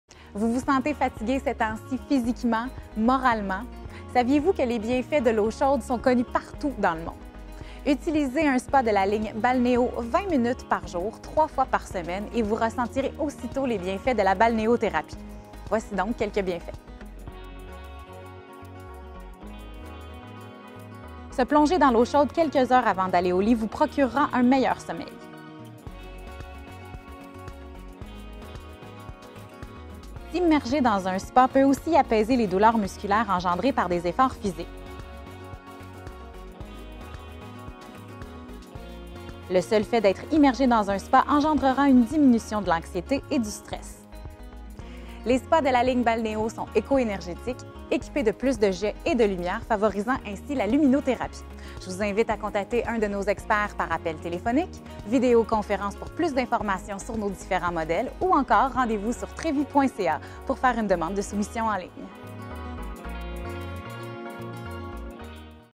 Quels sont les bienfaits de la balnéothérapie
Avec les spas de la ligne Balnéo Trévi, profitez de nombreux bienfaits sur votre santé physique et mentale.